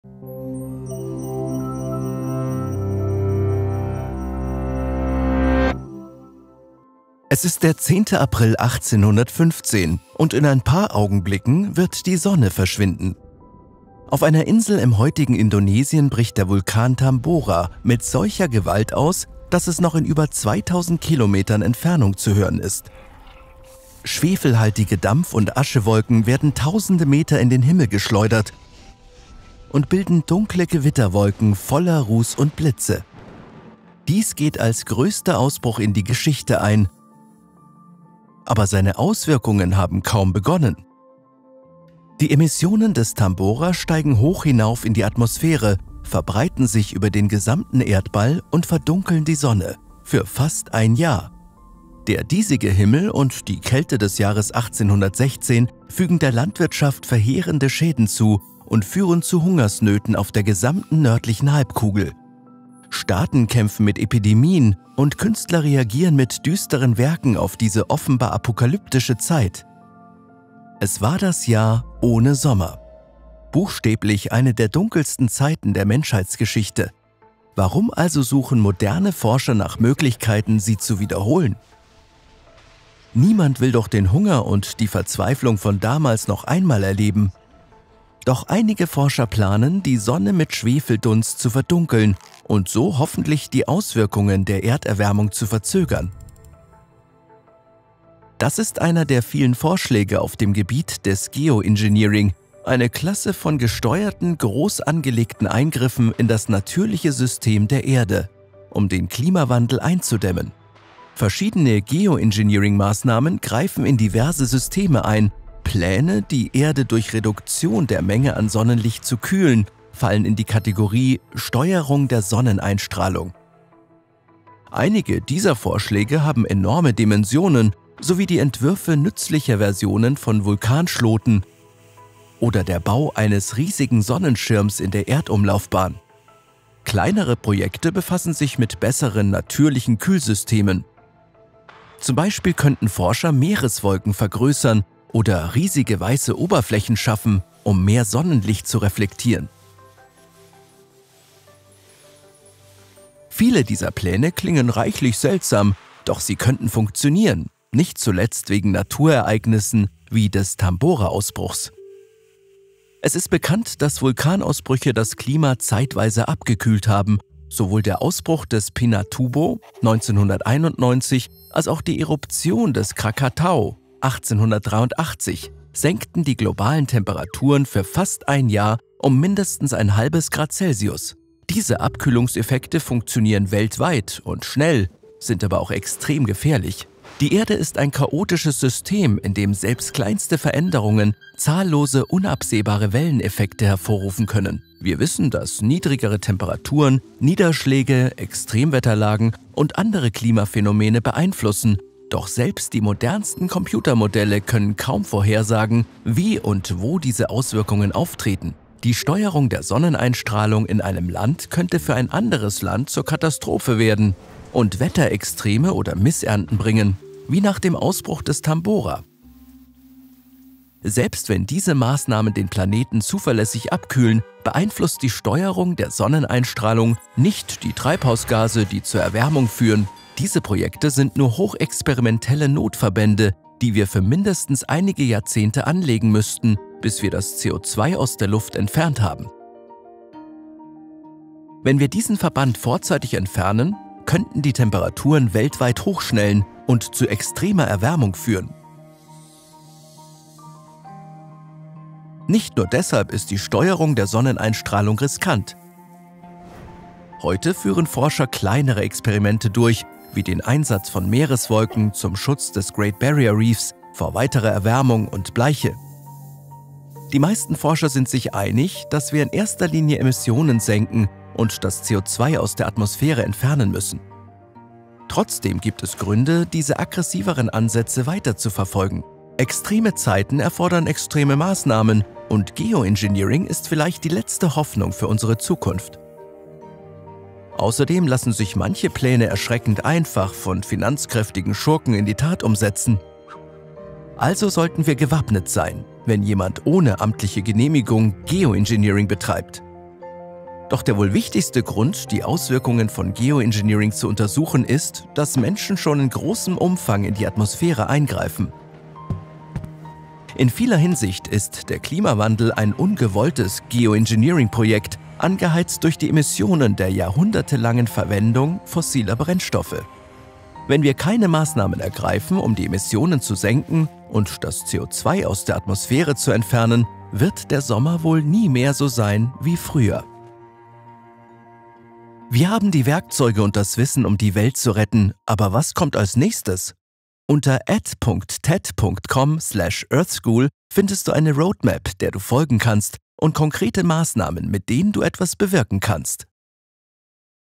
Das Jahr ohne Sommer - David Biello
Im Jahr 1815 brach der Berg Tambora aus, und seine Emissionen verbreiteten sich über den gesamten Globus, so dass die Sonne fast ein ganzes Jahr lang verdeckt war. Dies hatte verheerende Auswirkungen auf die Landwirtschaft und führte zu Hungersnöten in der gesamten nördlichen Hemisphäre. Es war das Jahr ohne Sommer - eine der dunkelsten Perioden in der Geschichte der Menschheit. Warum also erwägen einige moderne Forscher, dies zu wiederholen? David Biello geht dem Geoengineering auf den Grund.

Lektion von David Biello, regie von Léon Moh-Cah, Andi Concha, Na Na Na Studio.